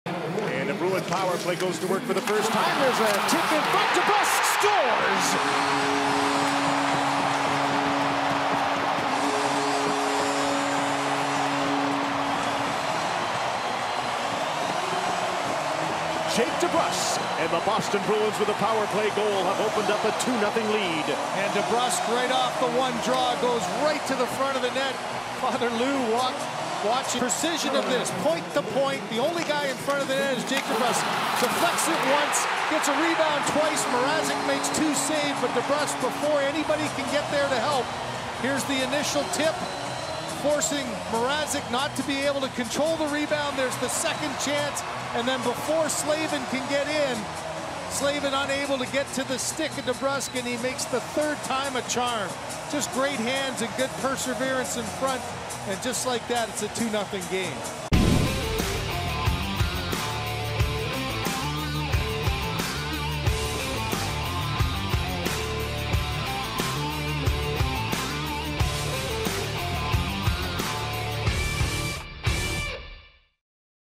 Jake DeBrusk Extends Bruins Lead To Delight Of Father Louie
Watch as Jake DeBrusk gives the Boston Bruins a 2-0 lead over the Carolina Hurricanes in Game 2 of the Eastern Conference Finals.